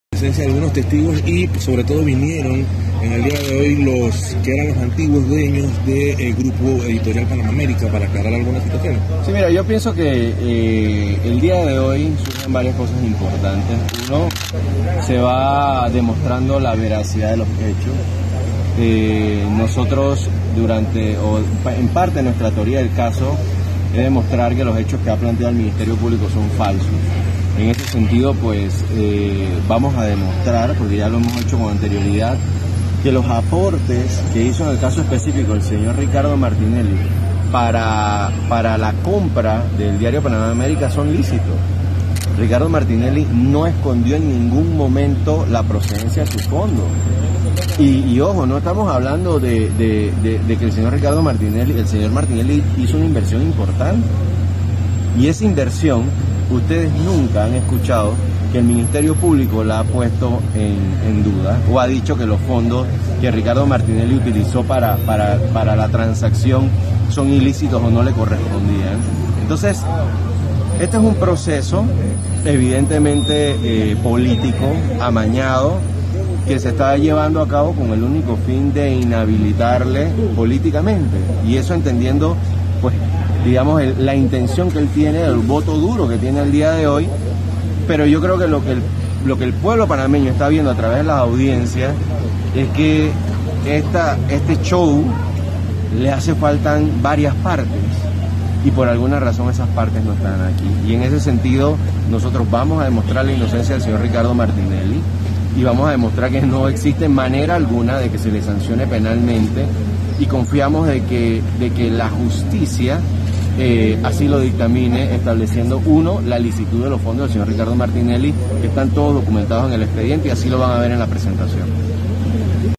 ESTO ES UN PROCESO POLÍTICO, AMAÑADO Y CON EL ÚNICO FIN DE INHABILITAR A RICARDO MARTINELLI
El abogado Luis Eduardo Camacho afirmó que demostrarán que los aportes que hizo @rmartinelli para la compra de Editora Panamá América son lícitos.
Martinelli no escondió en ningún momento la procedencia de sus fondos, que no han sido puestos en duda.

DIARIO: CRÍTICA
VÍA: @victorarosemena

✅ Ricardo Martinelli cuenta con una gran estructura y también tiene al TEAM RM 🏆

El pueblo quiere que VUELVA @rmartinelli